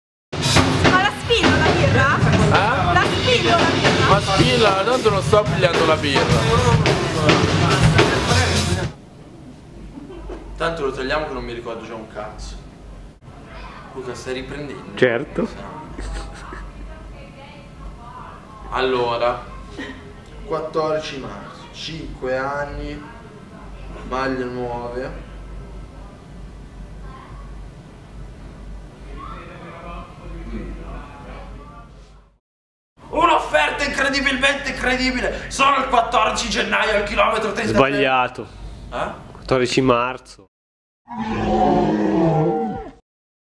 BRM NIGHT 2015 - il promo del PROMO #2
In attesa di svelare il Trailer definitivo e la line-up del concerto, eccovi una breve pillola di ciò che è successo sul set. Segnate in rosso sul calendario il 14-03-15, al KM33, via Mazzini 62, Trezzo sull'Adda (MI).
Starring: Mistery Boobs & Gianluca Beretta.